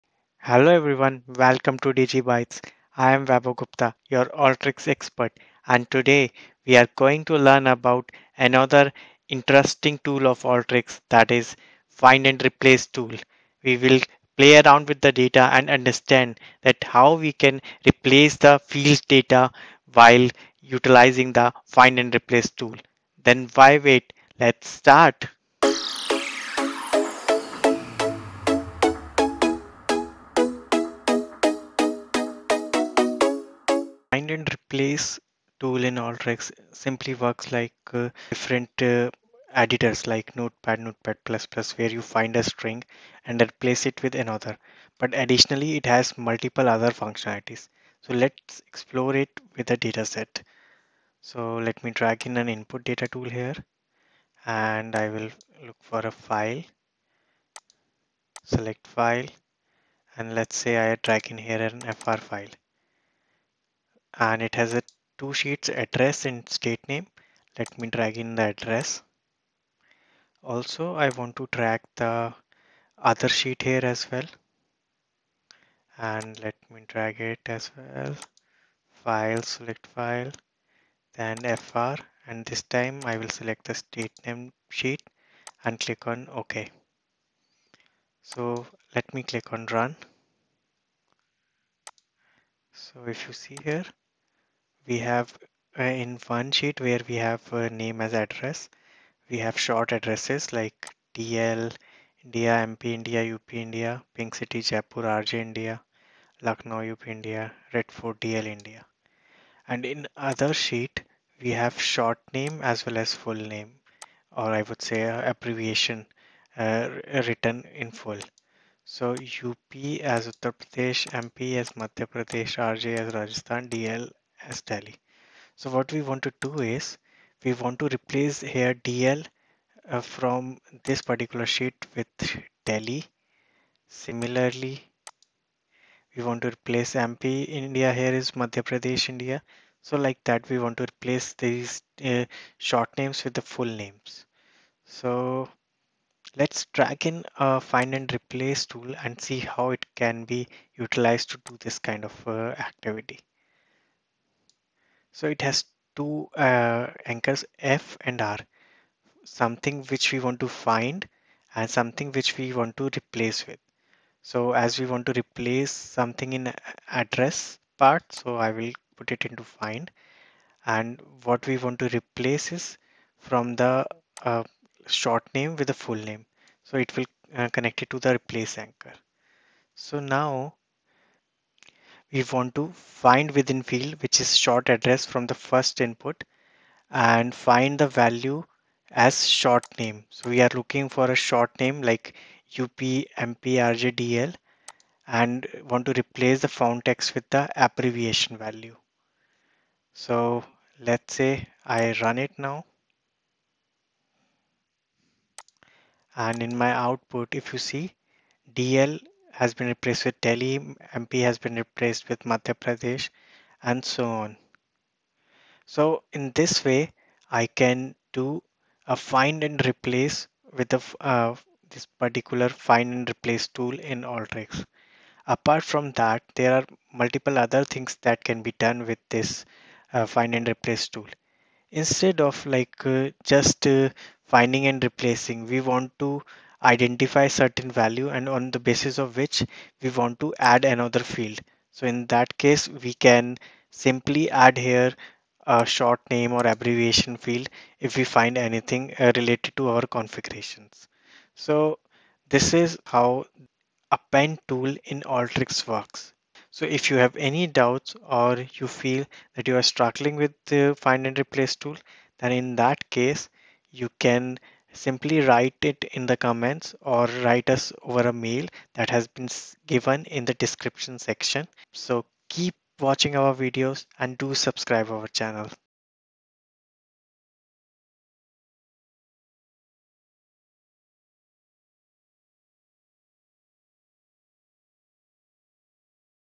Learn Alteryx Find & Replace Tool | Vaibhav Gupta
In this video, Vaibhav Gupta is explaining the use of Find & Replace tool of Alteryx with example. He is covering different configurations of the tool in detail.